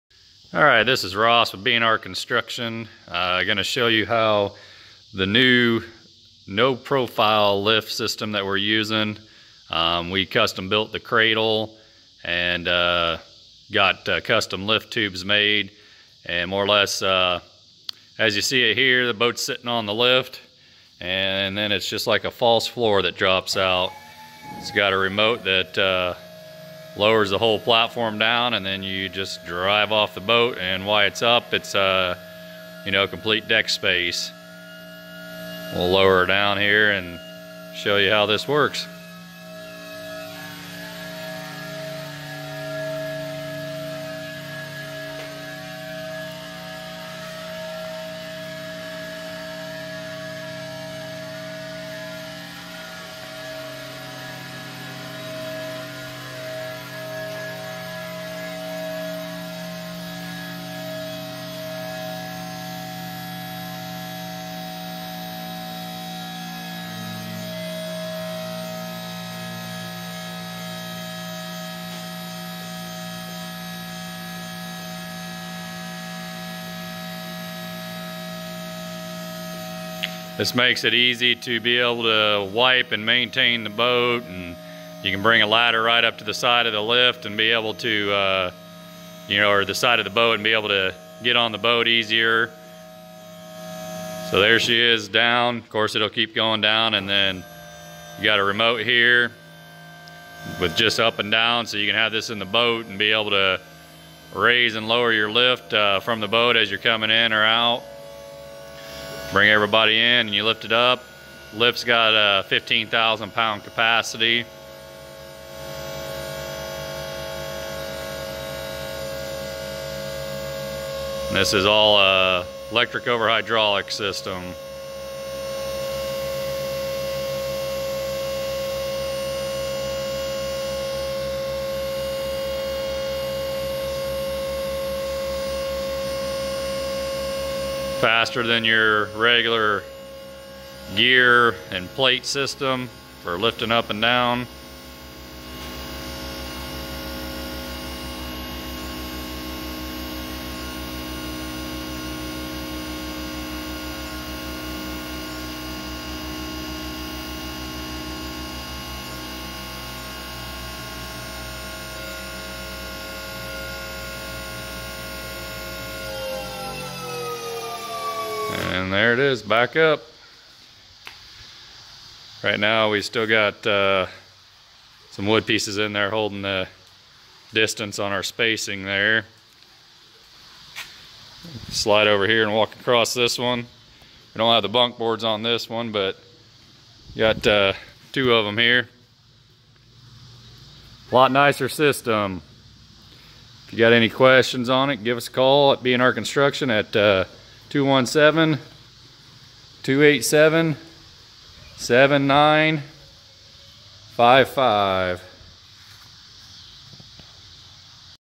Custom boat lift Custom boathouse Custom boat dock.
B&R Construction built a custom no profile boat lift. Give us a call today for a price.